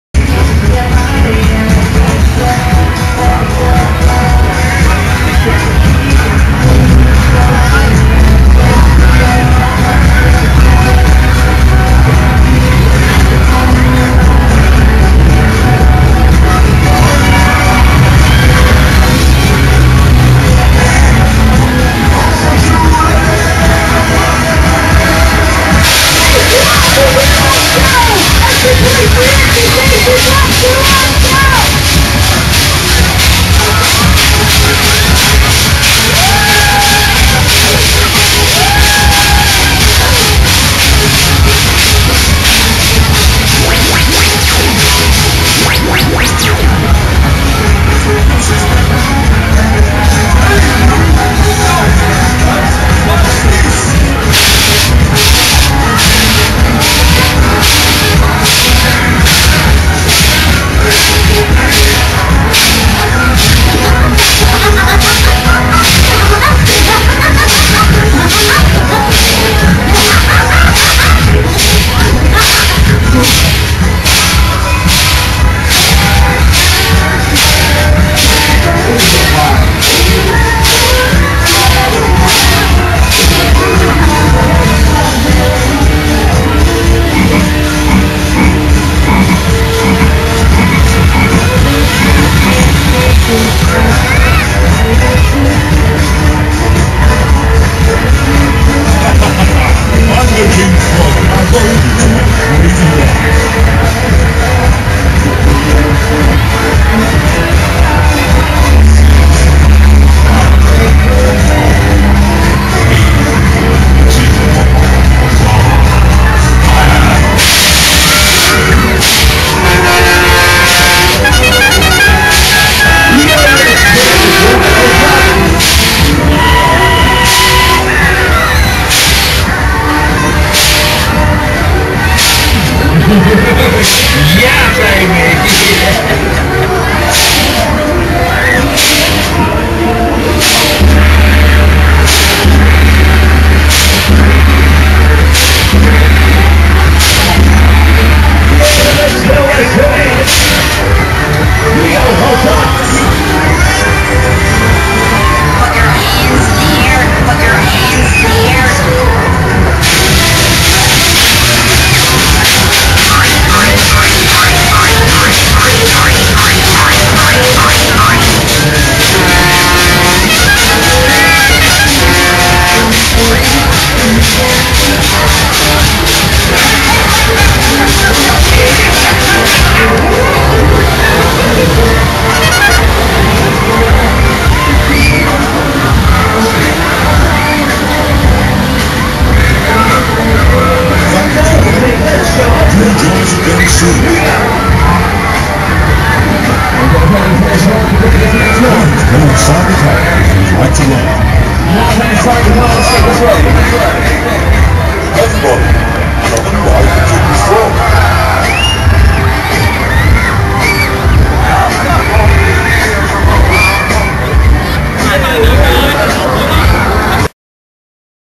King Frog
At the Wirral Show, my girlfriend & one of our mates went on the "King Frog" ride while I held the bags.